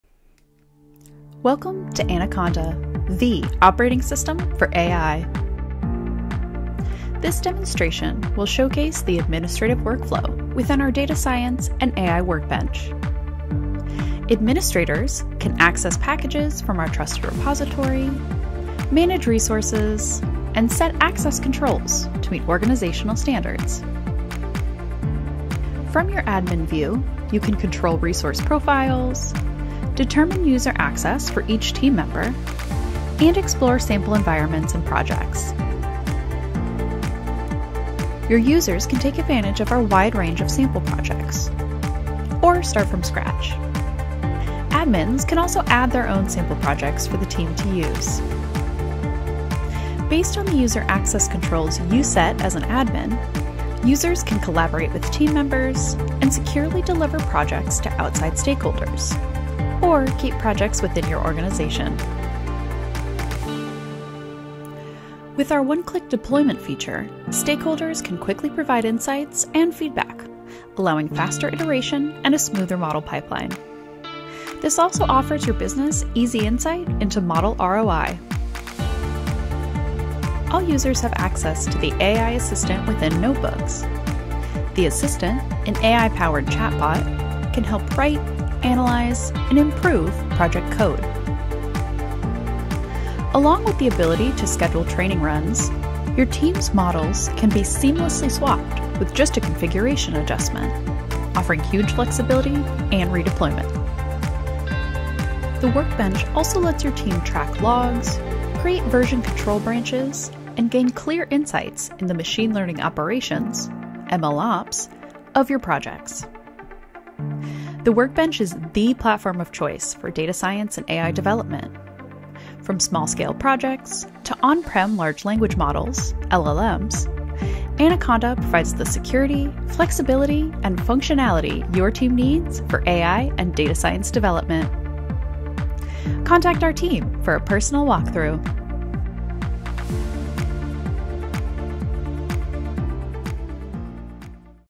Data Science & AI Workbench
Anaconda’s Data Science & AI Workbench is the all-in-one-platform for building business value with AI. From small scale projects to On-Prem Large Language Models (LLMs), the Workbench allows a secure, scalable solution for team collaboration and model deployment.